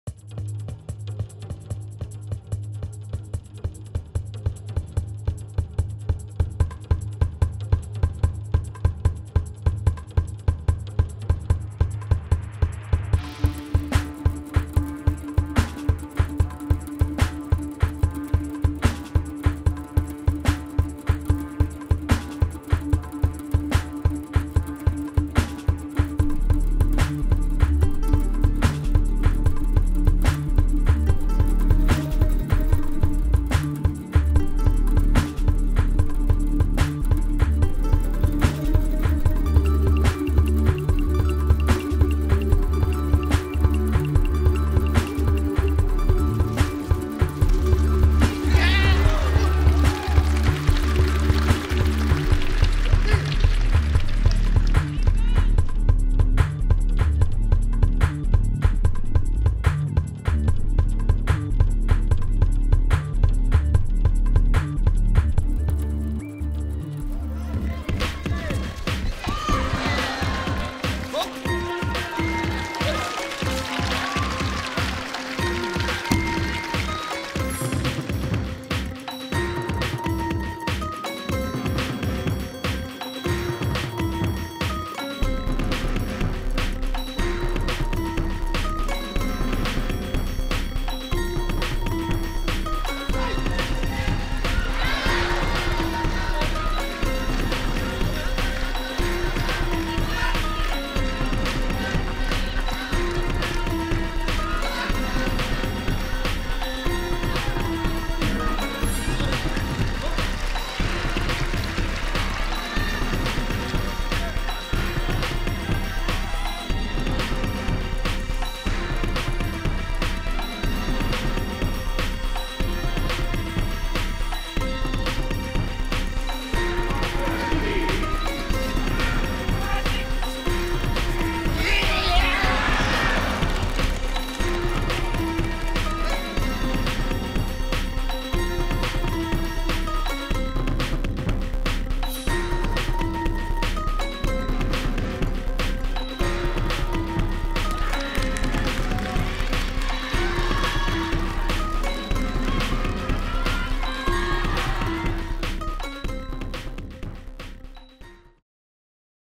Masahi Ebinuma (-66Kg) Day 1 Tokyo Grand Slam 2014
After a dominant display to reach the semi finals, triple World Champion Ebinuma will have to fight for bronze. Young up-and-comer Abe stuns his Japanese teammate and will fight for gold at -66kg!